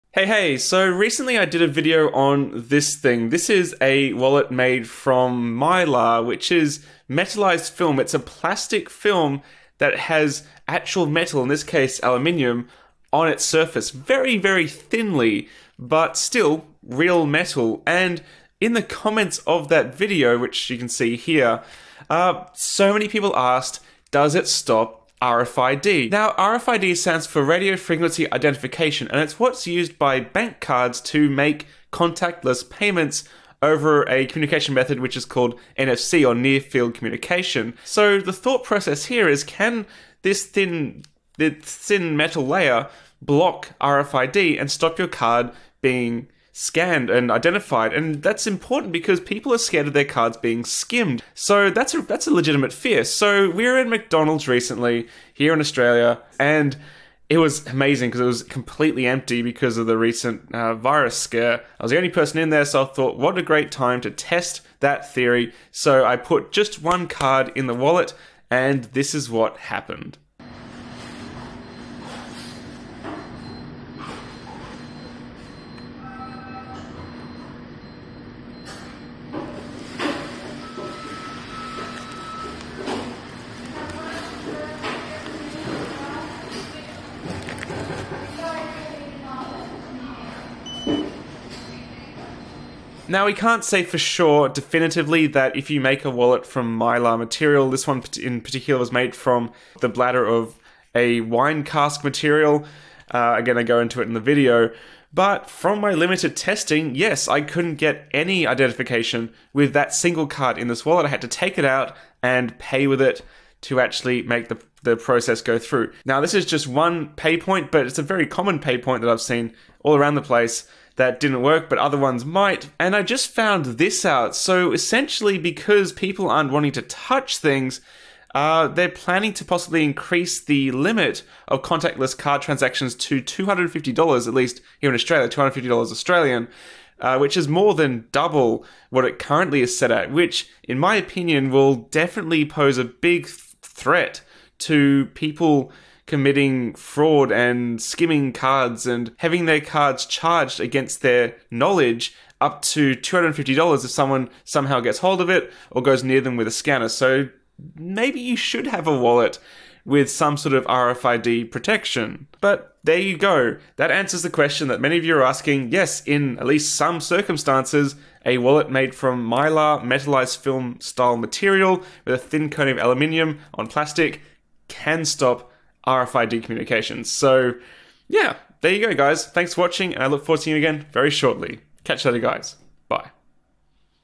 Can a Mylar Wallet Block RFID?
You asked, so I tested! This video represents my findings in testing the ability for a metallised film wallet to block contactless payment. It may not work in your case, or in different circumstances.

3D Printing Essentials